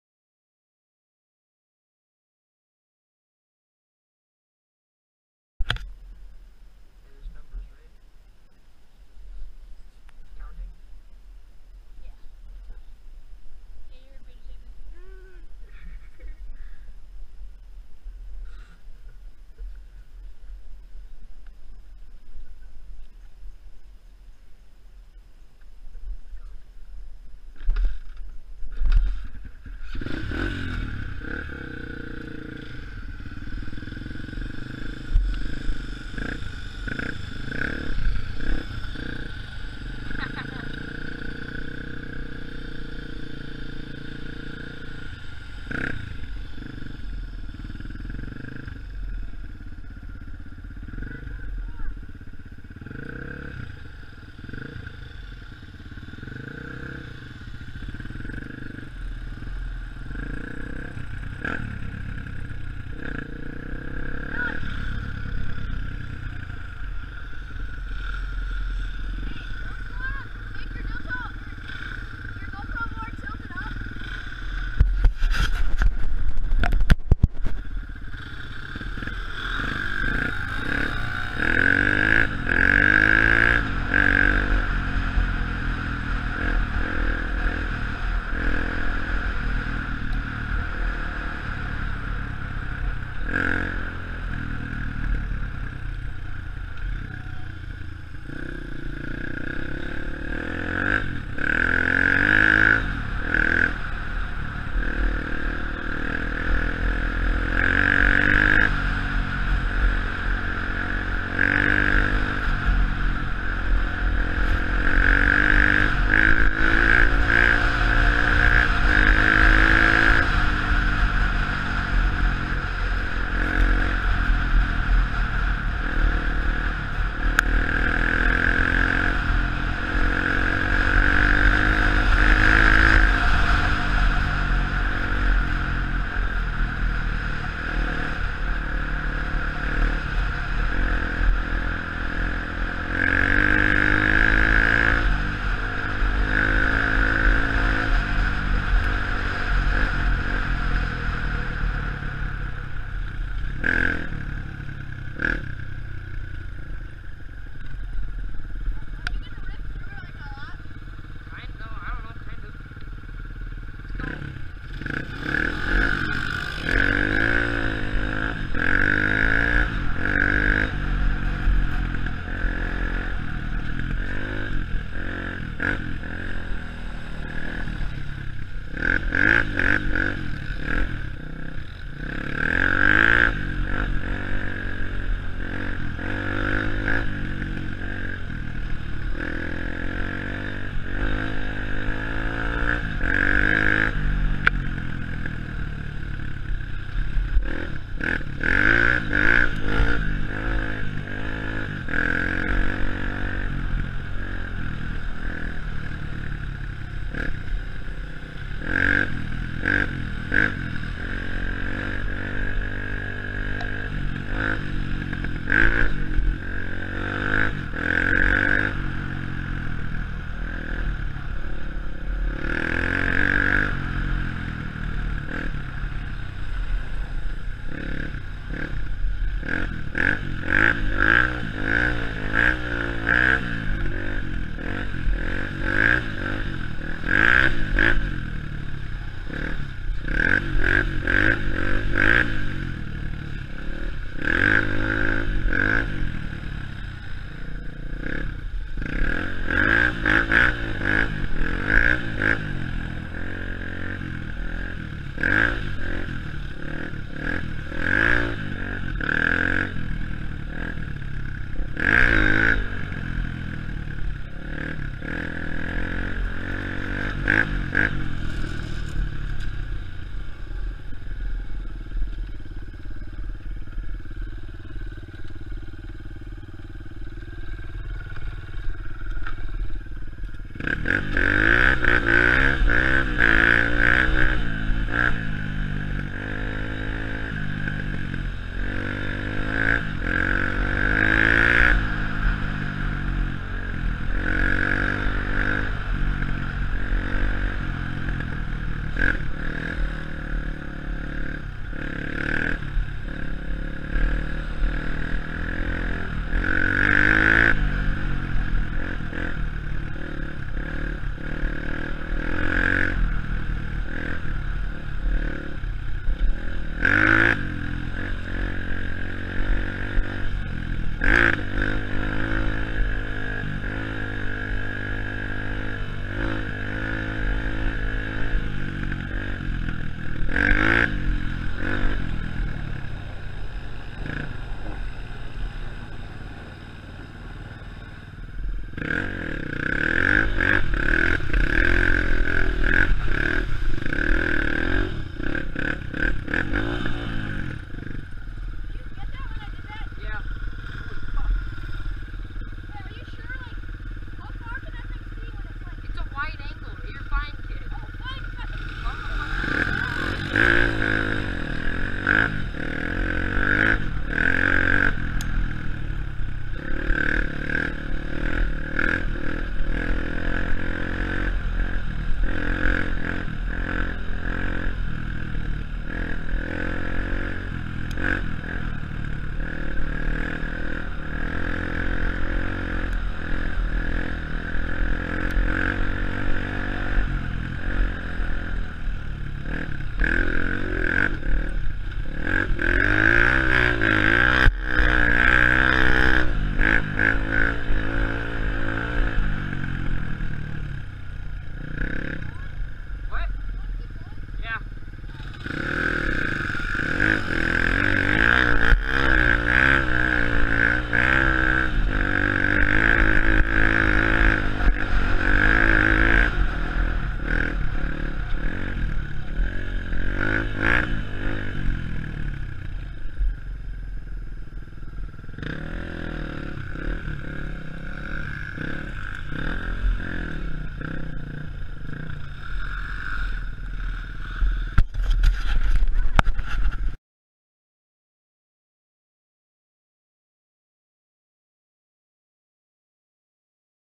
Dirt Biking and Four Wheeling in Minnesota
Messing around in Finlayson, Minnesota. Honda crf150f. Polaris pheonix 200.